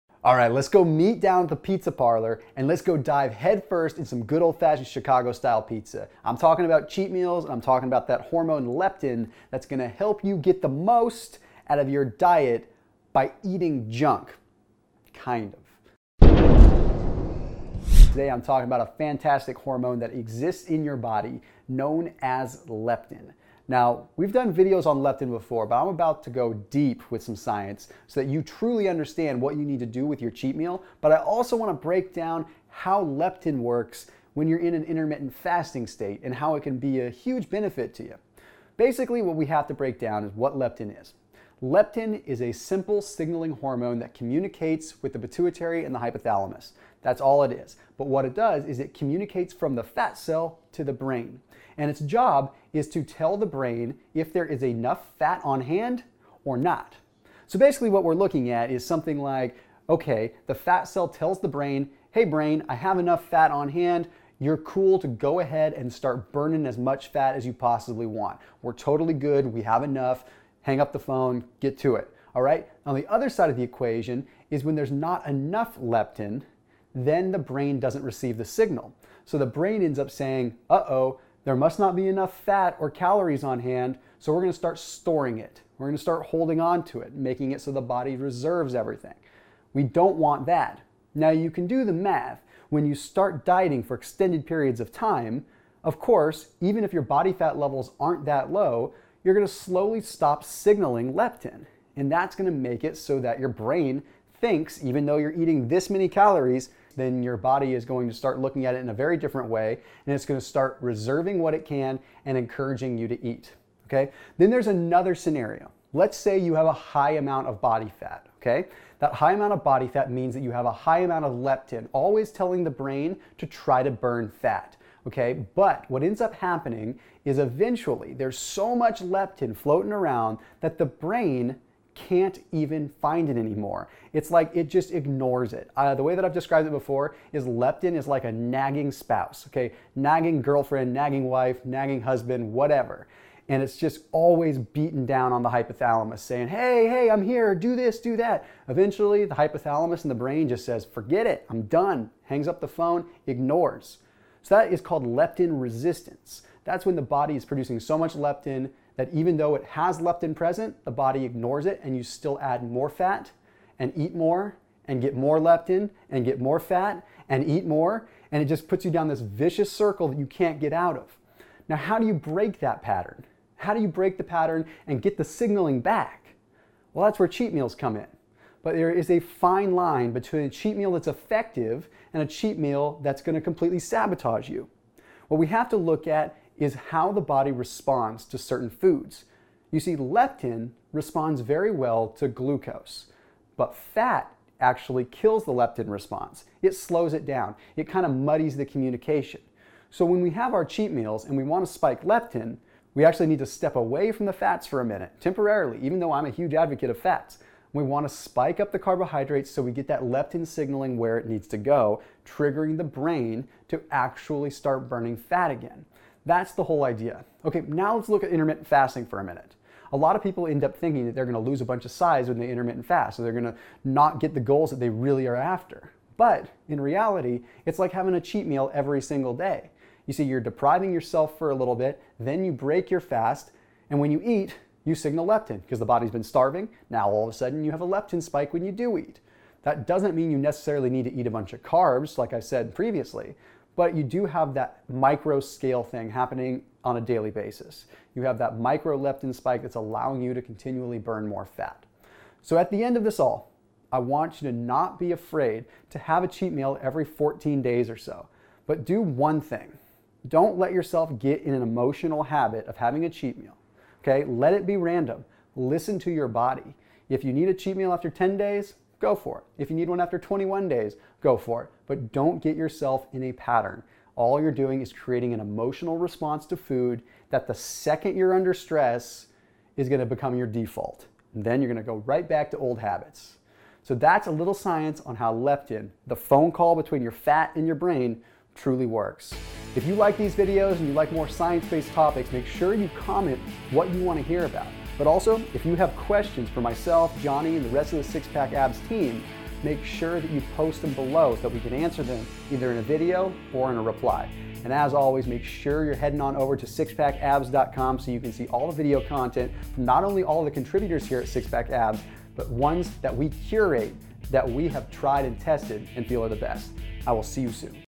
The Truth About Cheat Meals - How To Use Cheat Meals To Boost Leptin And Burn Fat
Let's meet up at the pizza parlor and dive head-first into a delicious, Chicago deep-dish pizza... I'm talking about cheat meals and utilizing your hormone Leptin to get the most out of your diet from eating junk...Kind of...

Thomas Delauer here today and I'm about to go deep into breaking down your hormone leptin to understand what you do with your cheat meals and how leptin works when you're doing intermittent fasting. Let's get to it:

0:41 - What is leptin? What does leptin do?

1:32 - Dieting for extended periods of time and its impact on leptin levels

1:54 - Being in a high body fat percentage and your leptin levels

2:15 - Leptin is like a nagging girlfriend or spouse

2:30 - "Leptin Resistance"

2:51 - How do you break out of leptin resistance? Cheat meals?!

3:08 - Leptin responds well to glucose. Fat kills leptin response.

3:20 - When you have your cheat meal, step away from the fats - spike up carbohydrates instead

3:37 - Intermittent fasting and leptin

4:12 - Don't be afraid to have a cheat meal every 14 days or so. BUT, don't follow a pattern. Listen to your body.

There you have it guys - your way to utilize cheat meals to your advantage to burn more fat and get into the best shape yet.

-Thomas